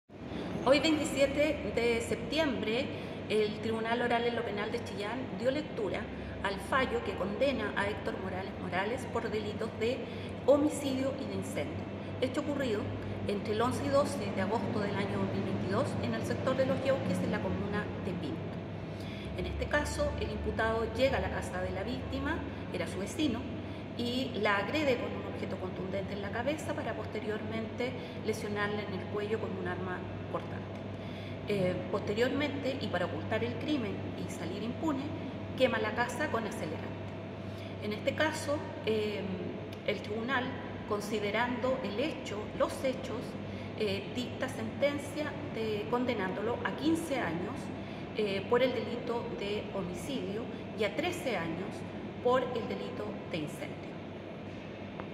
Condenan a sujeto que mató a mujer e incendio casa en Pinto
REGIONAL. - El Tribunal Oral en lo Pena de Chillán dictó las penas de 15 y 13 años de presidio para el hombre que mató a una mujer y luego incendió la casa para encubrir el crimen ocurrido en Pinto.

Héctor Morales Morales (43), dio muerte a María Inés Sepúlveda Matus (63) y luego incendió la casa de ella para borrar las evidencias, hecho ocurrido entre la noche del 11 y madrugada del 12 de agosto de 2022, en la comuna de Pinto.

Los hechos se registraron al interior de una vivienda, ubicada en el kilómetro 52 de la Ruta N-55, en el sector Los Lleuques de la mencionada comuna, hasta donde llegó el ahora condenado, quien era vecino de la víctima.

El acusado consiguió que la mujer le abriera la puerta de la cocina. Una vez dentro del inmueble, y mientras la víctima estaba de espaldas, la golpeó con un elemento contundente en la cabeza y ya en el suelo la apuñaló, lo que le causó la muerte.

Luego, llevó el cuerpo al dormitorio y lo dejó en la cama, para rociar con parafina y bencina el colchón, y prender fuego. Producto de esto, se incendió toda la vivienda, según relató la fiscal Maritza Camus.

La fiscal Maritza Camus destacó el trabajo del Laboratorio de Criminalística (Labocar) y del Departamento OS9, ambos de Carabineros, cuyos peritajes, y demás medios de prueba, permitieron que los magistrados del TOP de Chillán lograran la convicción sobre la participación y culpabilidad de Héctor Morales Morales.